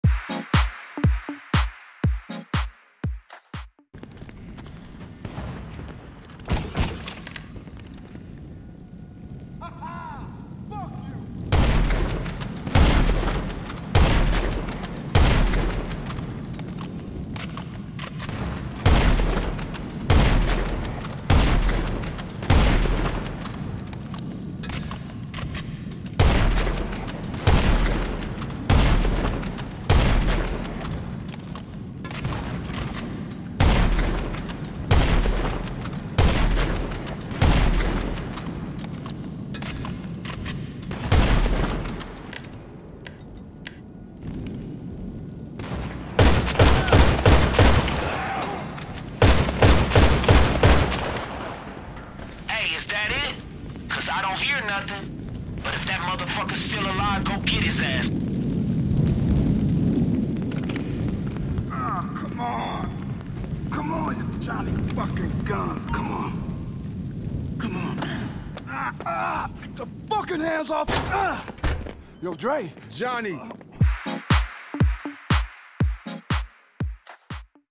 Johnny Guns is EXPLOSION-Proof
He can survive explosions, just not a handful of other rounds.